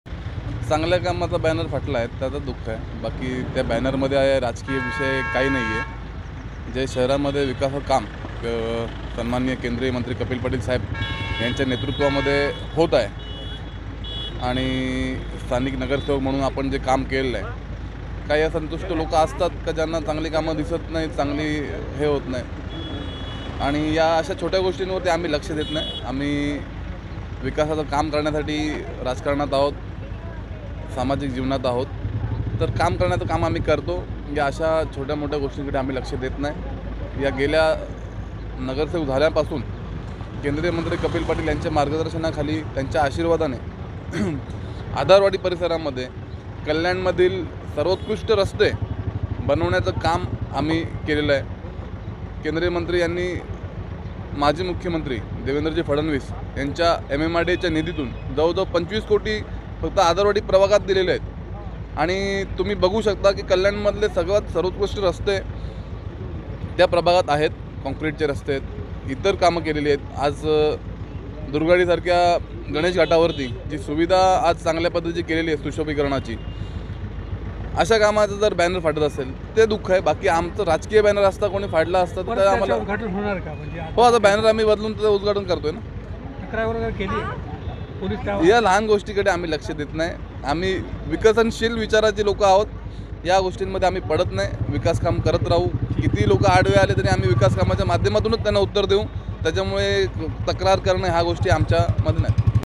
कल्याण : केंद्रीय मंत्री कपिल पाटील यांच्या उद्घाटनआधी केंद्रीय मंत्र्यांचे बॅनर फाडले ....
दुर्गाडी गणेश घाटावरील केंद्रीय मंत्री कपिल पाटील यांचे बॅनर कुणी अज्ञात इसमाने फाडल्याने भाजप कार्यकर्ते पदाधिकाऱ्यांमध्ये संतापाचे वातावरण होते. रविवारी केंद्रीय मंत्री कपिल पाटील यांच्या हस्ते खासदार निधीतून साकारण्यात आलेल्या कल्याण मधील विविध वास्तूंचे उद्घाटन व लोकार्पण करण्यात येणार होते..दुर्गाडी गणेश घाट परिसरात सुशोभीकरनाच्या कामाचे लोकार्पण व भूमिपूजन आज केंद्रीय पंचायतराज मंत्री कपिल पाटील यांच्या हस्ते करण्यात येणार होते . मात्र त्यापूर्वीच कुणी अज्ञात इसमाने त्याच्या उद्घाटनचा बॅनर फाडल्याचे समोर आले आहे . याप्रकरणी स्थानिक माजी नगरसेवक पाटील यांनी  चांगल्या कामाचे बॅनर फाडले याचं दुःख आहे .
कपिल पाटील यांच्या हस्ते आज कल्याण मधील विविध विकास कामांचा लोकार्पण होणार आहे .त्यांनी कल्याण मधील रस्त्यांसाठी कोट्यवधीचा निधी दिलाय... विकास कामे होतायत त्यामुळे काही विघ्नसंतोशी असंतुष्ट लोकांनी हे कृत्य केलंय असा आरोप केला.